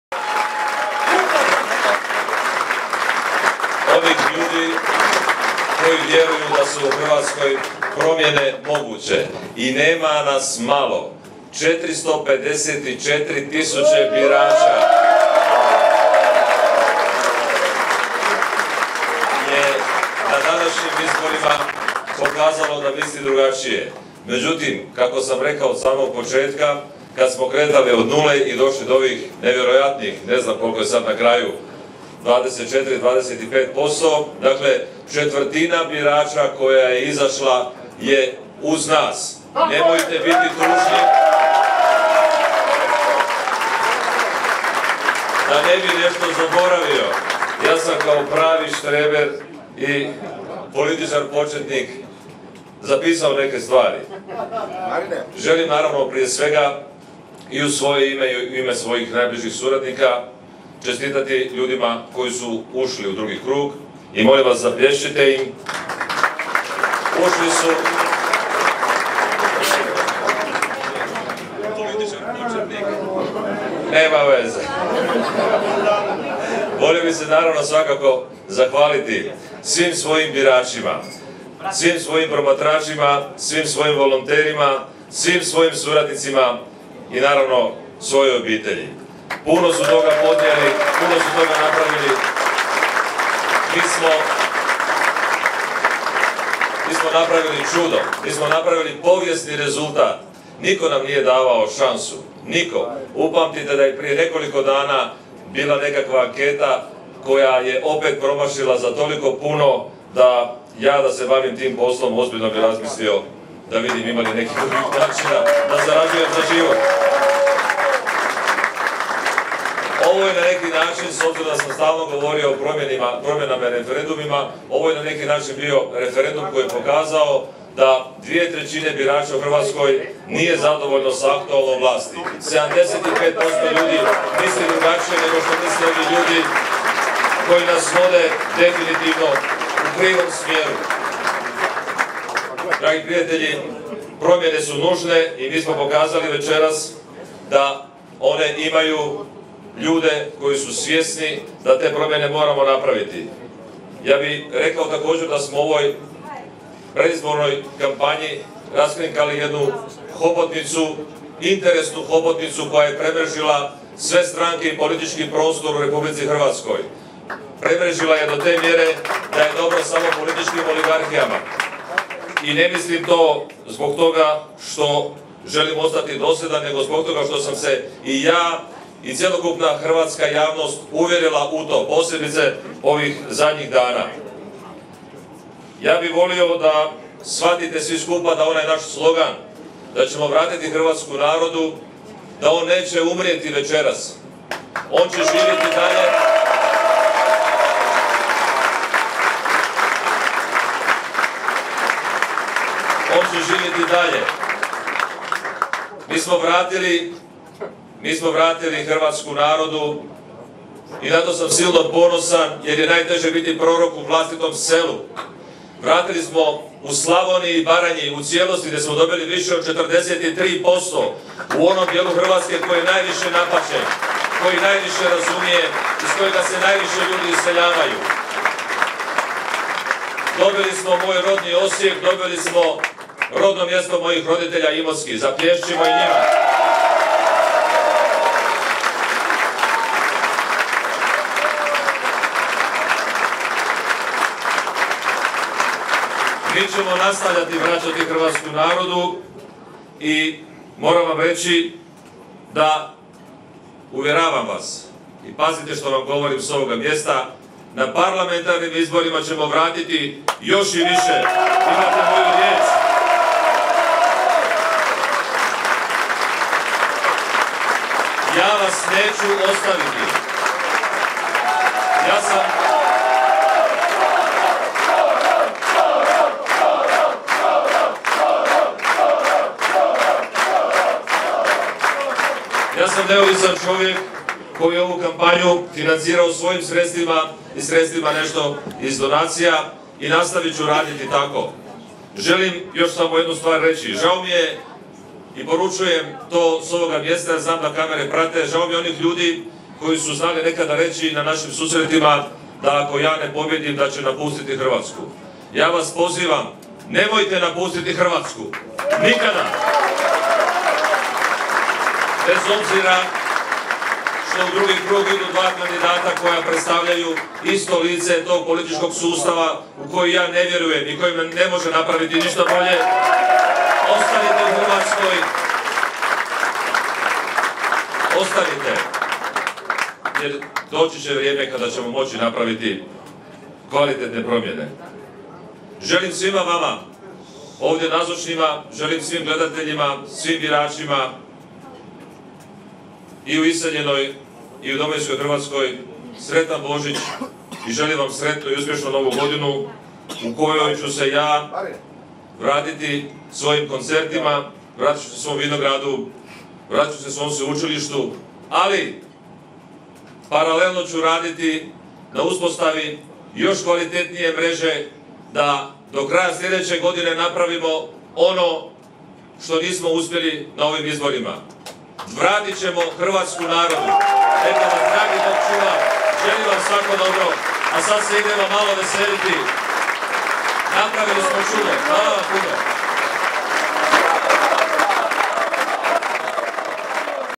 Obraćanje: Miroslav Škoro
"U ovom stožeru se večeras slavi pobjeda slobode izbora onih ljudi koji vjeruju da su u Hrvatskoj promjene moguće. I nema nas malo, 454.000 birača je na današnjim izborima pokazalo da misli drukčije! Četvrtina birača koja je izašla je uz nas, nemojte biti tužni!", poručio je večeras Škoro svojim pristašama u izbornom stožeru.